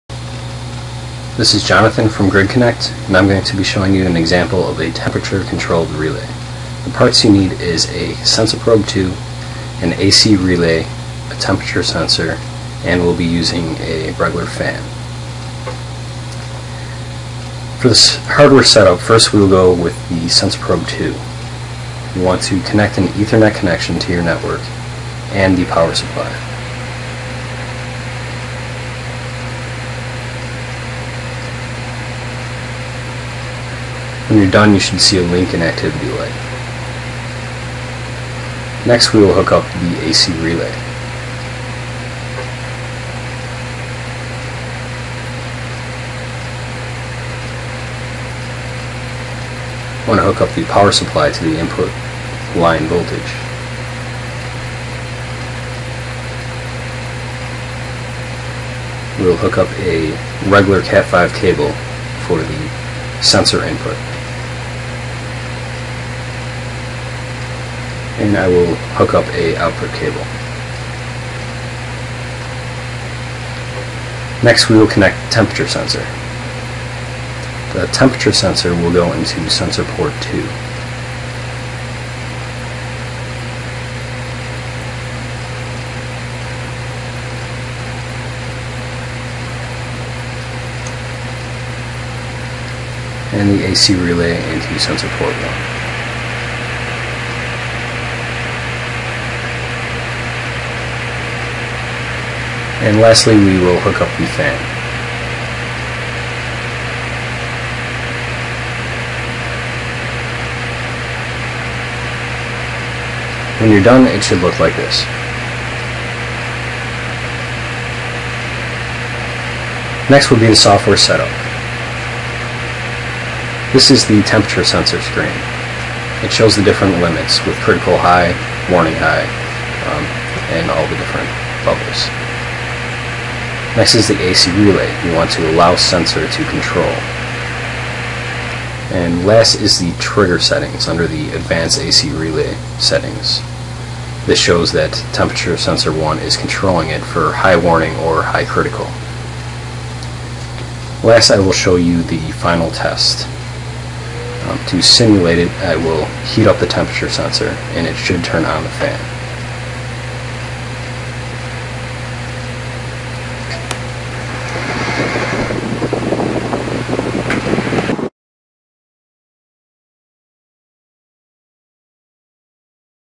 Sensor Probe 2 - AC Relay
This video demonstrates an application of our AC Relay sensor.